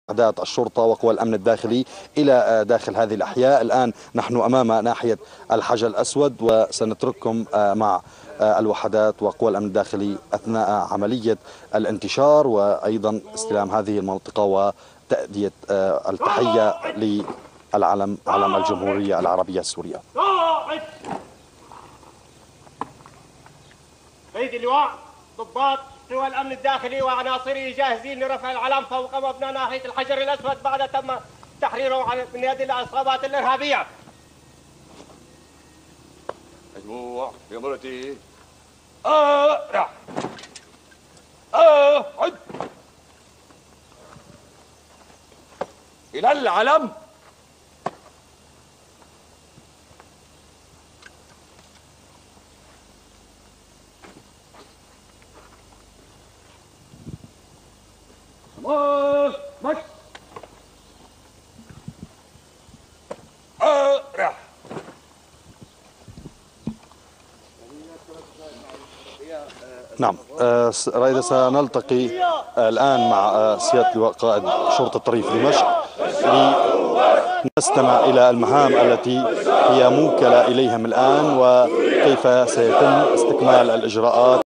Raising the Syrian Flag over the Town Hall in Al-Hajar Al-Aswad in Southern Damascus
Following the defeat of ISIS in Al-Hajar Al-Aswad, Syria law enforcement entered the neighborhood and raised the Syrian flag over the town hall.